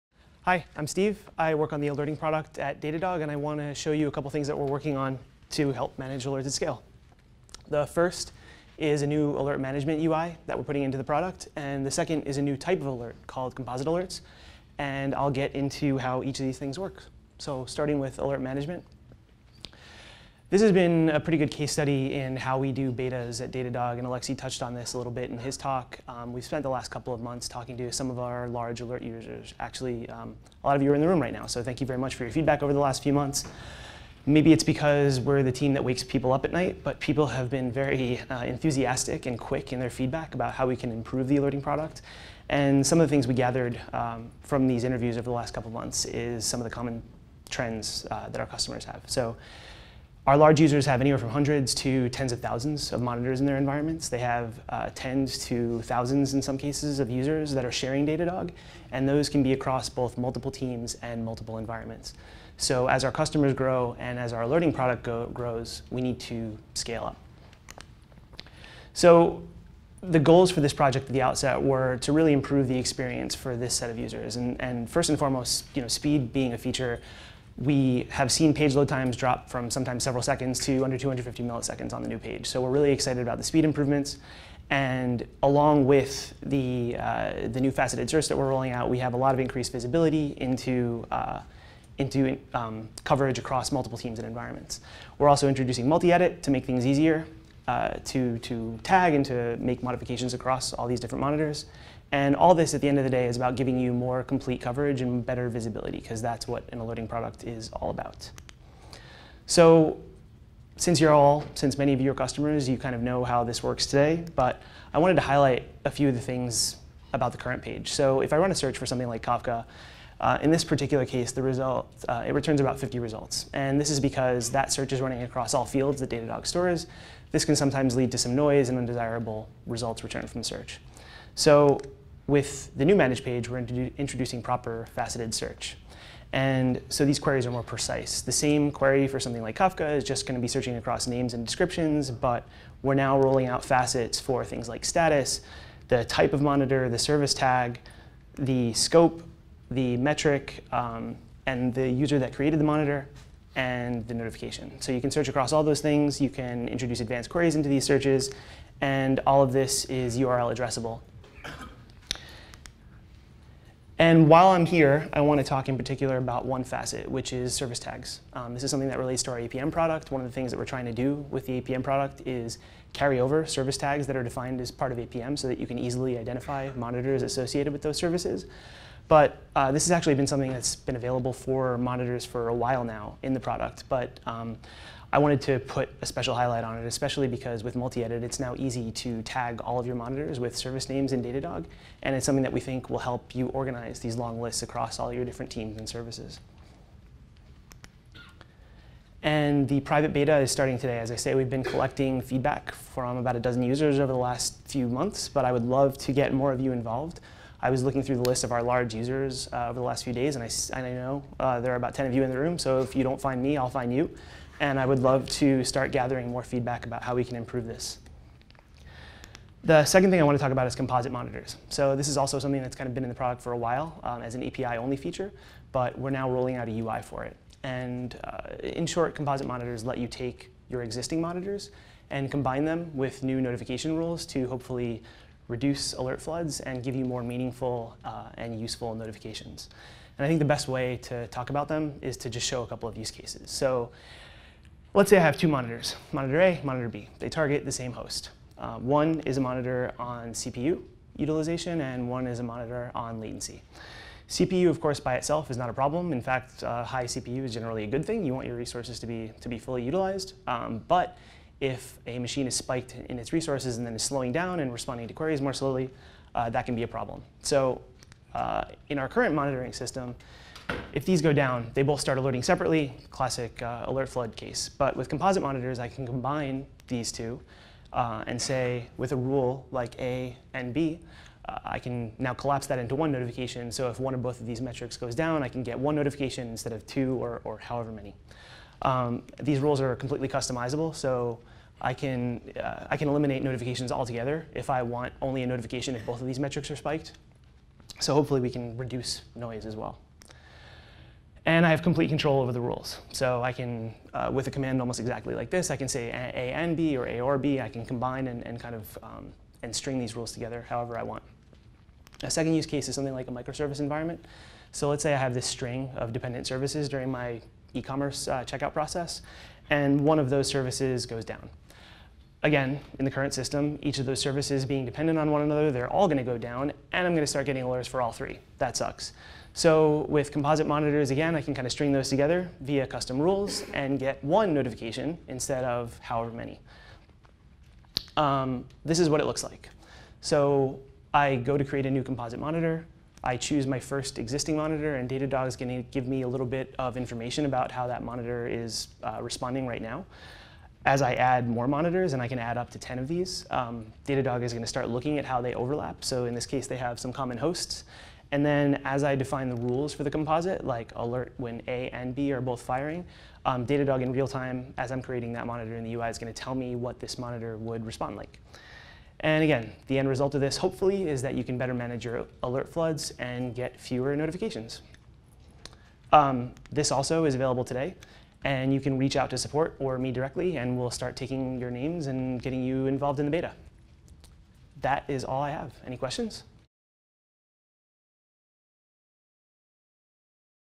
Managing alerts at scale - Stephen Boak
Stephen Boak demonstrates new tooling that Datadog has released for scaling your monitors and alerts These features are currently available via an open beta and you can request they be enabled for your organization by contacting .